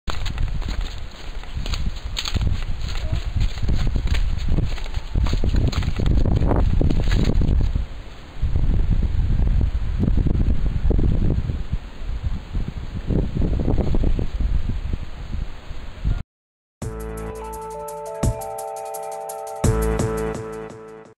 De jur împrejur- noroi
8 martie 2011, Găuzeni, Șoldănești
cam în așa stare sunt drumurile în sat după ploi sau ierni. Iar acea fântână se află pe strada centrală a satului, de unde consună apă cel puțin 5 familii